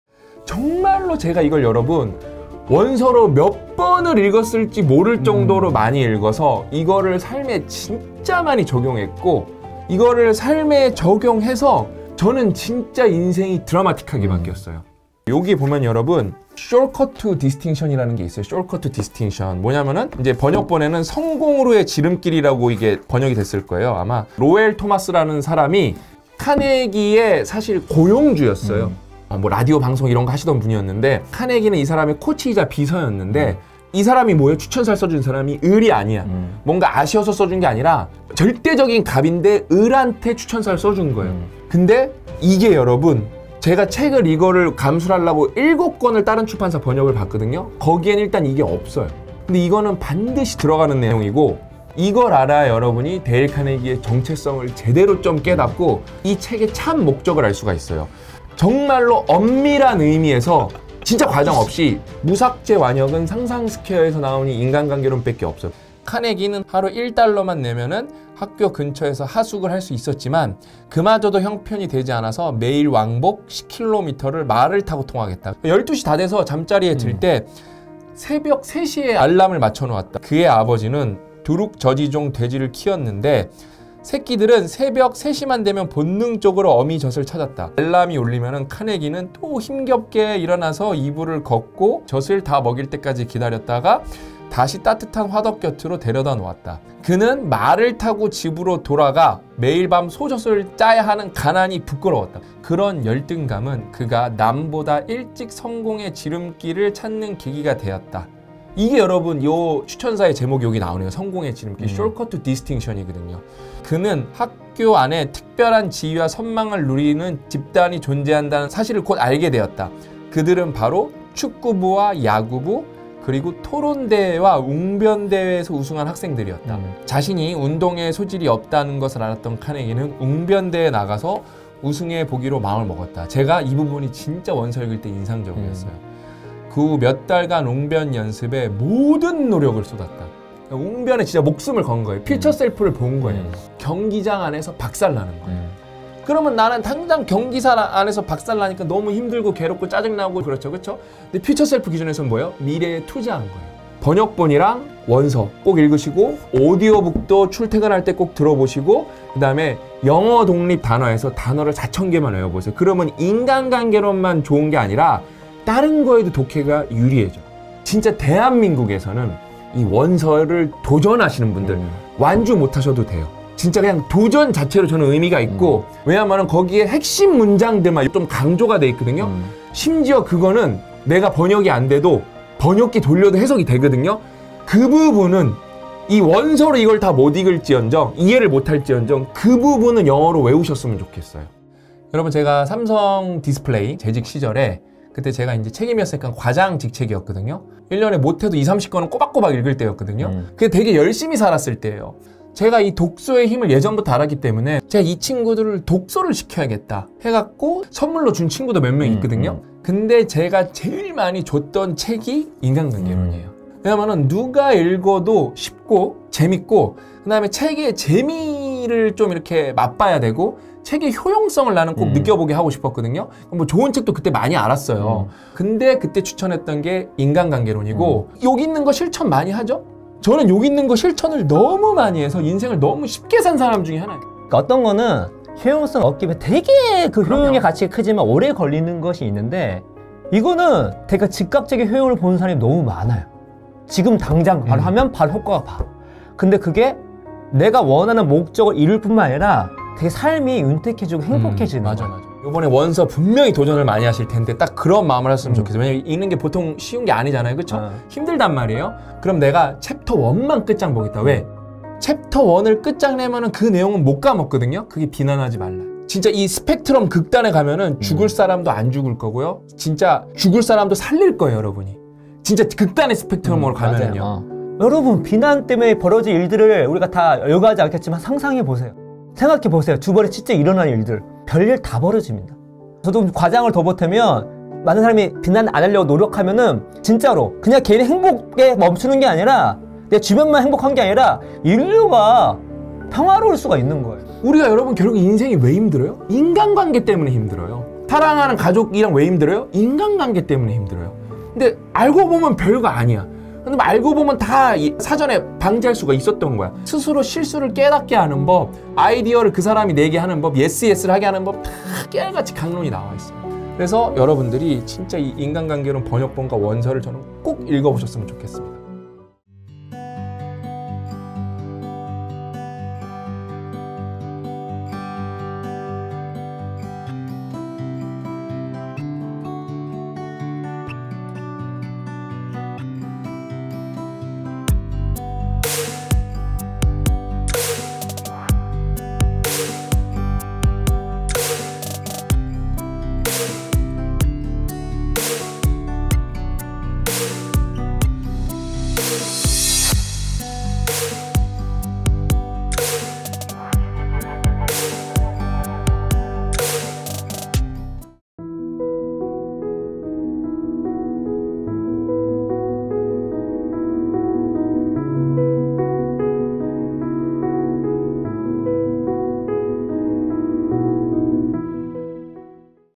성공으로 가는 지름길 확실하게 알려드립니다
사람의 마음을 사로잡는 방법에 관한 영원한 고전. 케네디, 오바마, 워런 버핏 등 이 시대의 리더들이 꼽은 인생 필독서《데일 카네기 인간관계론》

스터디언은 상상스퀘어의 미디어 브랜드입니다.